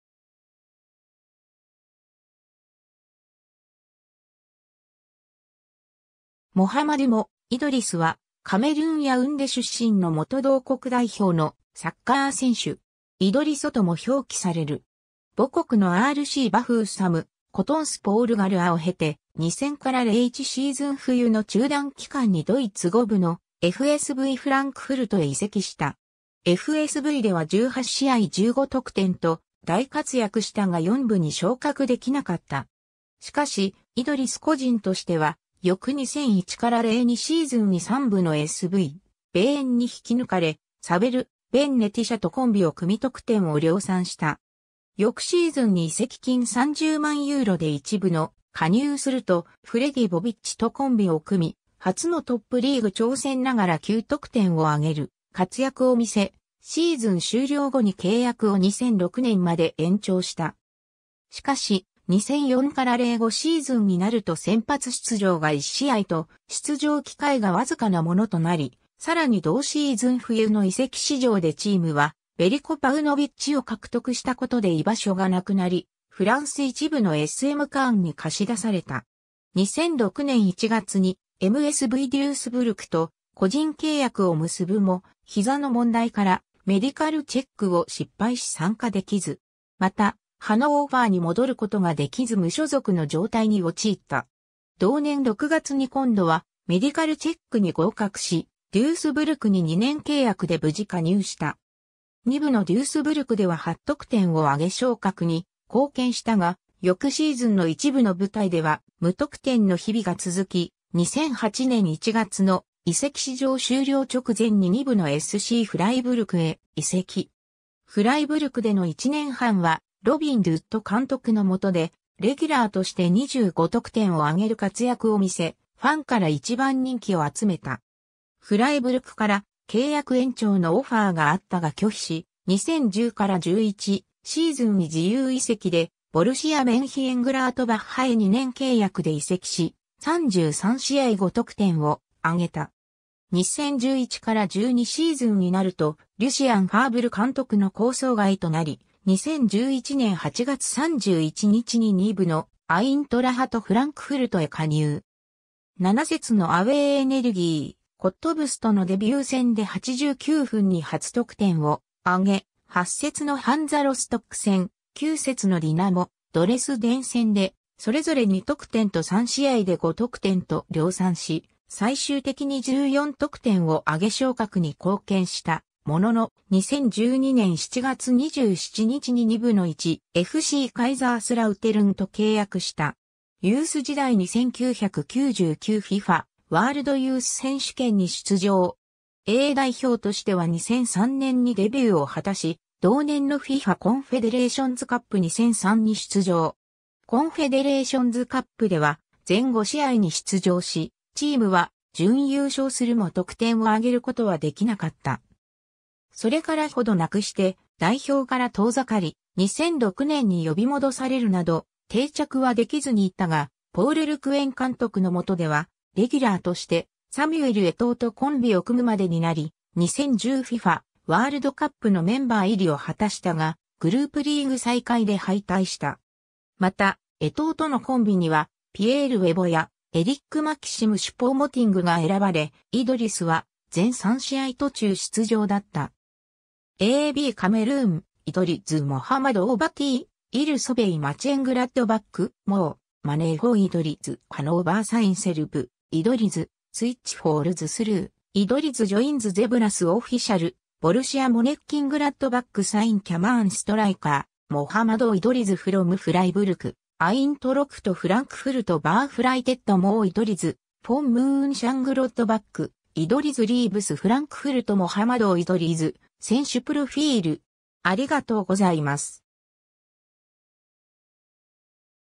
モハマドゥ・イドリス
コトンスポール・ガルアの選手
#1.FCカイザースラウテルンの選手
モハマドゥ "モ" ・イドリス（Mohammadou Idrissou, 1980年3月8日 - ）は、カメルーン・ヤウンデ出身の元同国代表のサッカー選手。
イドリソとも表記される。
 母国のRCバフーサム, コトンスポール・ガルアを経て、2000-01シーズン冬の中断期間にドイツ5部のFSVフランクフルトへ移籍した。
FSVでは18試合15得点と大活躍したが4部に昇格出来なかった。
しかし、イドリス個人としては翌2001-02シーズンに3部のSVヴェーエンに引き抜かれ、サベル・ベン・ネティシャ（Saber Ben Neticha）とコンビを組み得点を量産した。
 翌シーズンに移籍金30万ユーロで1部の加入すると、フレディ・ボビッチとコンビを組み、初のトップリーグ挑戦ながら9得点を挙げる活躍を見せ、シーズン終了後に契約を2006年まで延長した。
しかし、2004-05シーズンになると先発出場が1試合と出場機会が僅かなものとなり、さらに同シーズン冬の移籍市場でチームはヴェリコ・パウノヴィッチを獲得したことで居場所が失くなり、フランス1部のSMカーンに貸し出された。
 2006年1月にMSVデュースブルクと個人契約を結ぶも、膝の問題からメディカルチェックを失敗し参加出来ず。
また、ハノーファーに戻ることが出来ず無所属の状態に陥った。
同年6月に今度はメディカルチェックに合格し、デュースブルクに2年契約で無事加入した。
2部のデュースブルクでは8得点を挙げ昇格に貢献したが、翌シーズンの1部の舞台では無得点の日々が続き、2008年1月の移籍市場終了直前に2部のSCフライブルクへ移籍。
フライブルクでの1年半は、ロビン・ドゥット監督の下でレギュラーとして25得点を挙げる活躍を見せ、ファンから一番人気を集めた。
 フライブルクから契約延長のオファーがあったが拒否し、2010-11シーズンに自由移籍でボルシア・メンヒェングラートバッハへ2年契約で移籍し、33試合5得点を挙げた。
2011-12シーズンになるとリュシアン・ファーヴル監督の構想外となり、2011年8月31日に2部のアイントラハト・フランクフルトへ加入。
7節のアウェー エネルギー・コットブスとのデビュー戦（3-3）で89分に初得点を挙げ、8節のハンザ・ロストック戦（4-1）, 9節のディナモ・ドレスデン戦（4-1）でそれぞれ2得点と3試合で5得点と量産し、最終的に14得点を挙げ昇格に貢献したものの、2012年7月27日に2部の1.FCカイザースラウテルンと契約した。
 ユース時代に1999 FIFAワールドユース選手権に出場。
A代表としては2003年にデビューを果たし、同年のFIFAコンフェデレーションズカップ2003に出場。
コンフェデレーションズカップでは全5試合に出場し、チームは準優勝するも得点を挙げることは出来なかった。
それから程なくして代表から遠ざかり、2006年に呼び戻されるなど定着は出来ずにいたが、ポール・ル・グエン監督の下ではレギュラーとしてサミュエル・エトーとコンビを組むまでになり、2010 FIFAワールドカップのメンバー入りを果たしたが、グループリーグ最下位で敗退した。
また、エトーとのコンビにはピエール・ウェボやエリック・マキシム・シュポ＝モティングが選ばれ、イドリスは全3試合途中出場だった。
 ^ a b "Cameroun: Idrissou Mohamadou va-t-il sauver Mochengladbach?" ^ "Mo' money for Idrissou" ^ "Hannover sign Serb" ^ "Idrissou switch falls through" ^ "Idrissou joins Zebras" ^ "Official: Borussia Moenchengladbach Sign Cameroon Striker Mohamadou Idrissou From Freiburg" ^ "Eintracht Frankfurt verpflichtet Mo Idrissou von Mönchengladbach" ^ "Idrisso...